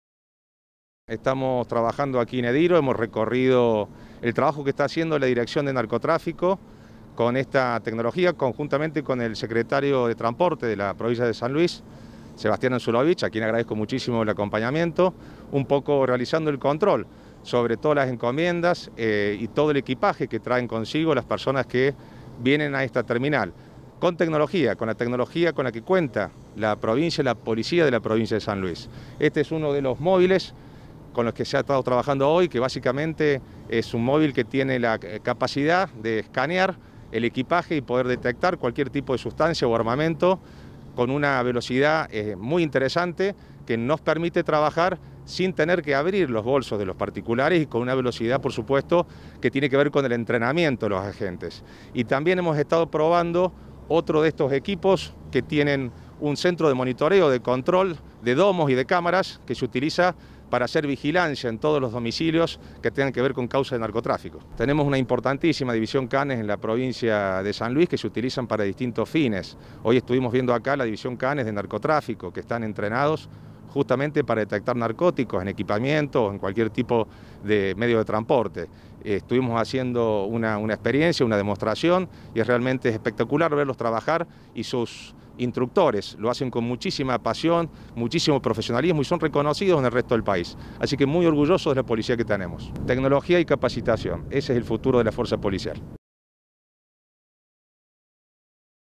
INVERSIÓN EN LA LUCHA CONTRA EL NARCOTRÁFICO
El ministro de seguridad de la provincia, acompañado del secretario de transporte, presentaron nuevo equipamiento para la división “Lucha contra el narcotráfico” de la policía de la provincia. El equipamiento consta de vehículos de última tecnología que recorrerán distintos puntos estratégicos.